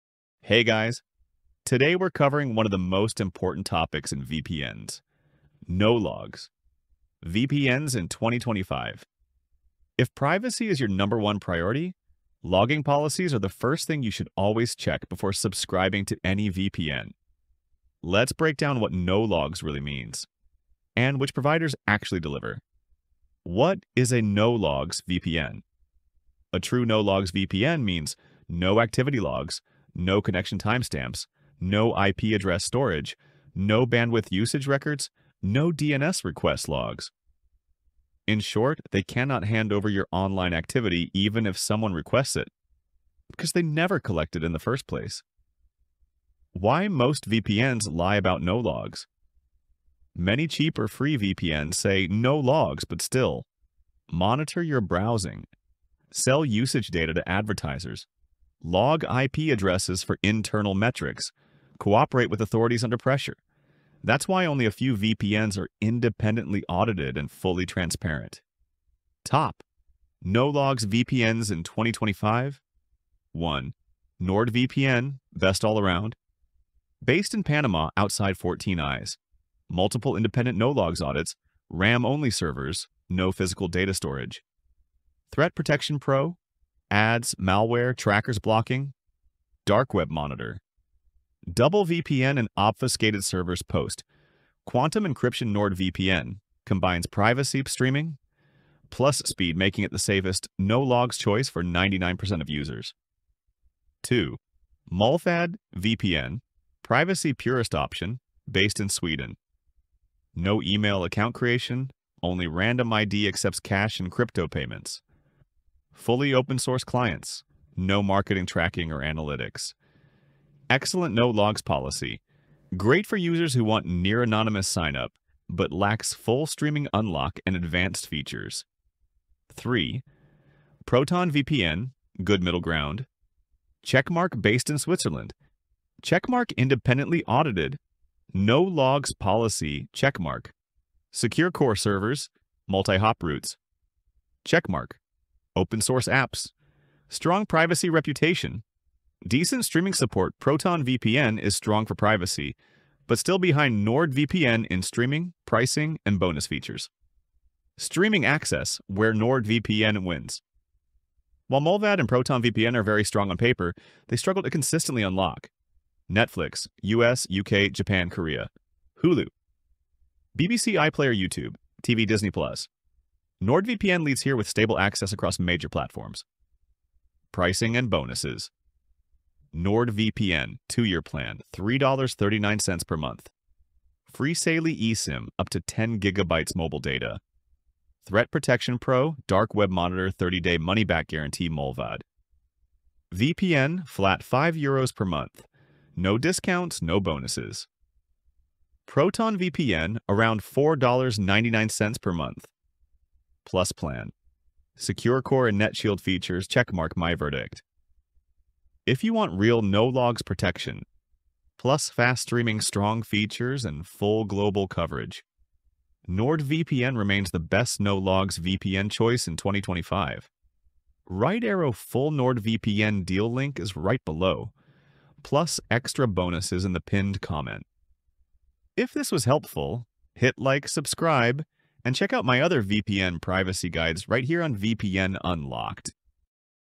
Best No-Logs VPNs 2025 — Which VPN Truly Keeps No Data?
Which VPNs truly keep no logs in 2025? I break down NordVPN, Mullvad, and ProtonVPN with full privacy comparisons, real audits, streaming access, and security features.

✅ Full no-logs VPN breakdown
✅ NordVPN, Mullvad, ProtonVPN privacy guide

🎁 Bonus deal also in the pinned comment!

📌 What you’ll learn today:
• No-logs VPN explained
• Which providers truly keep no data
• Streaming, security & pricing breakdown
• Honest verdict for 2025